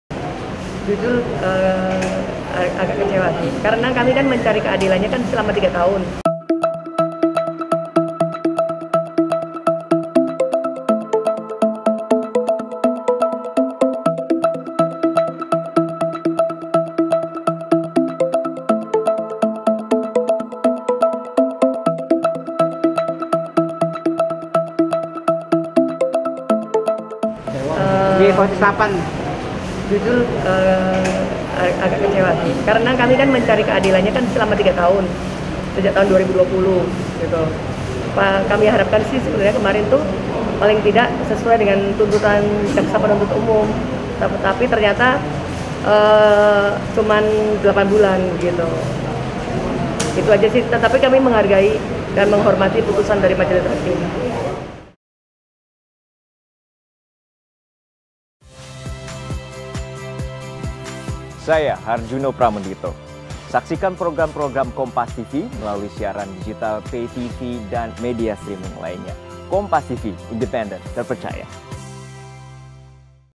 Korban Penipuan KSP Indosurya Kecewa Natalia Rusli Hanya Divonis 8 Bulan Penjara!
JAKARTA, KOMPAS.TV - Verawati Sanjaya, mantan klien Natalia Rusli mengungkapkan kekecewaannya usai Majelis Hakim Pengadilan Negeri (PN) Jakarta Barat memvonis terdakwa dengan pidana penjara selama 8 bulan.

Adapun Verawati merupakan korban investasi bodong Koperasi Simpan Pinjam (KSP) Indosurya, yang ditipu Natalia Rusli saat menangani kasus ini sebagai kuasa hukum. Verawati berpendapat bahwa vonis hakim terlalu ringan untuk Natalia.

"Jujur agak kecewa karena (vonis) terlalu ringan," ujar Verawati saat ditemui usai persidangan di PN Jakarta Barat, Selasa (20/6/2023).